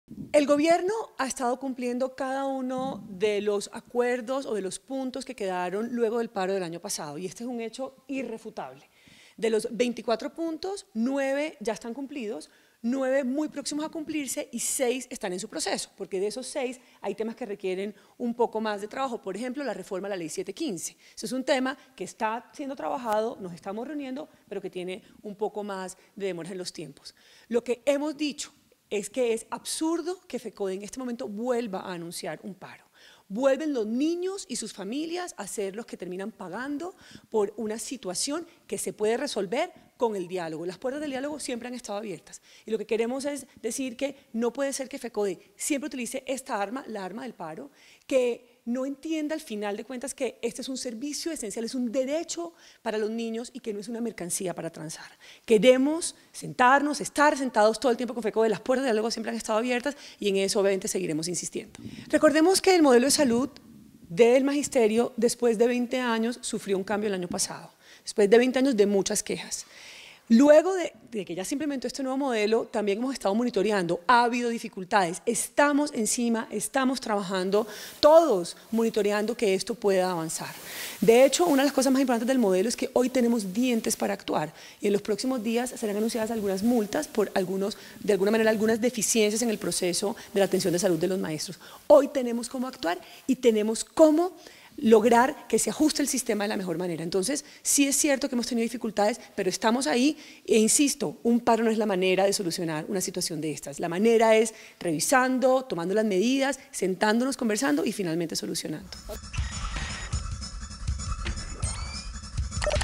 ‘Paro anunciado por Fecode es absurdo’: Yaneth Giha
La ministra de Educación, Yaneth Giha, calificó de absurdo el anuncio de Fecode de realizar un nuevo paro el próximo 9 y 10 de mayo debido a los incumplimientos de los acuerdos pactados con el Gobierno.